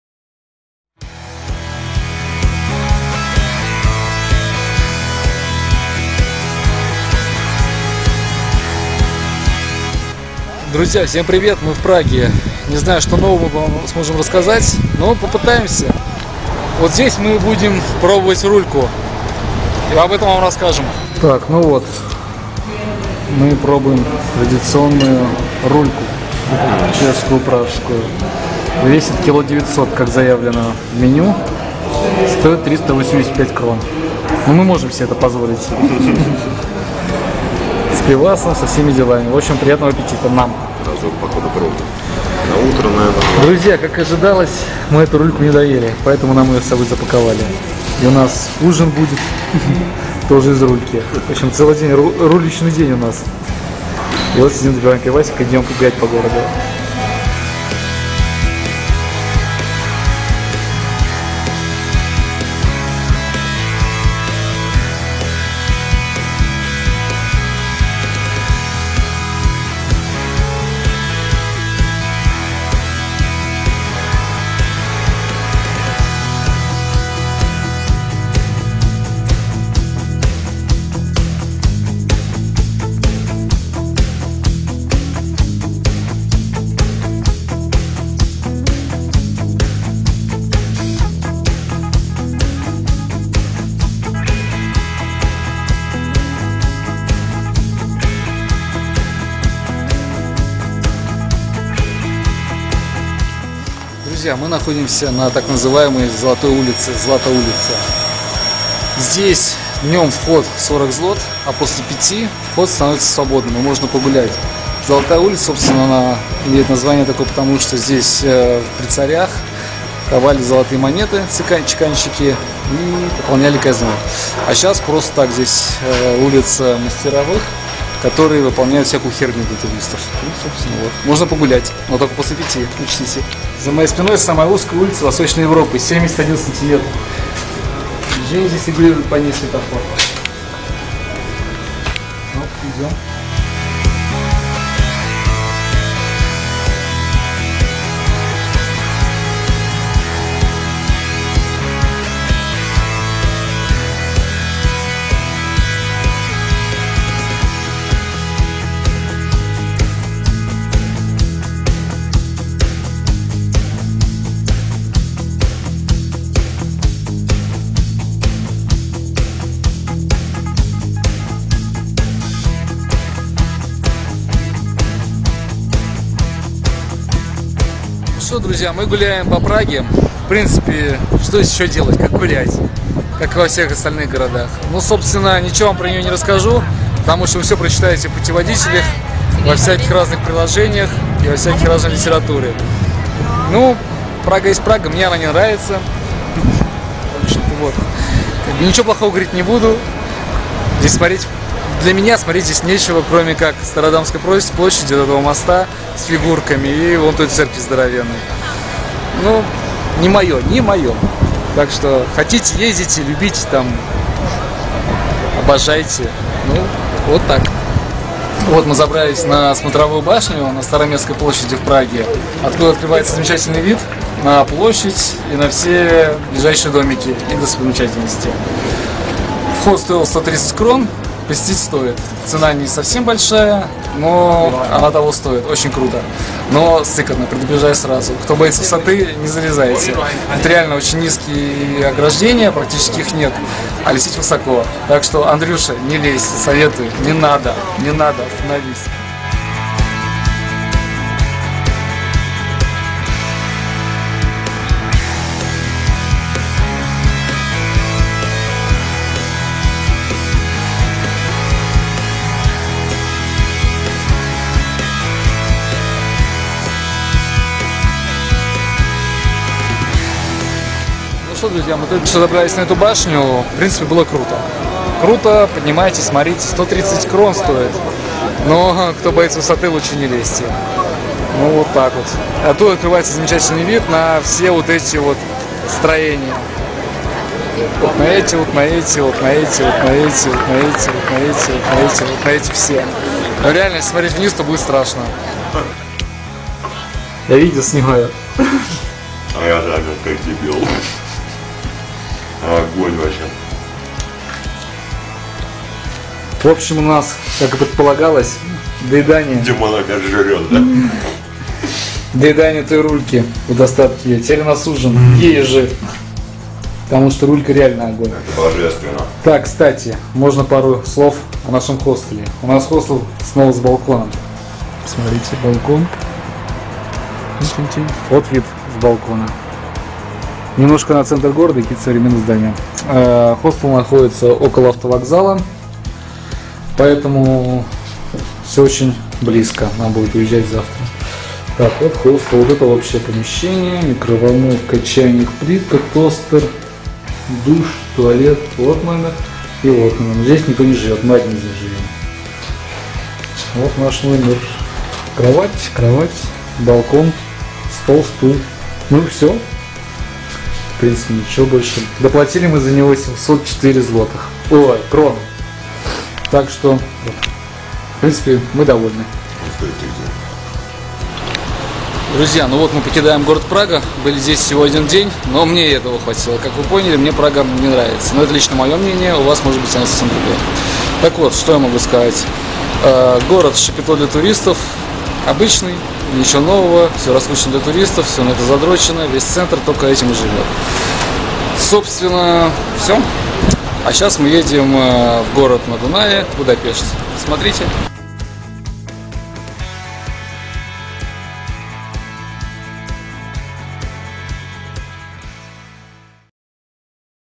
Чехия. Прага. Город-шапито для туристов
Шестой город на нашем пути в этом Евротрипе.
30 мая 2016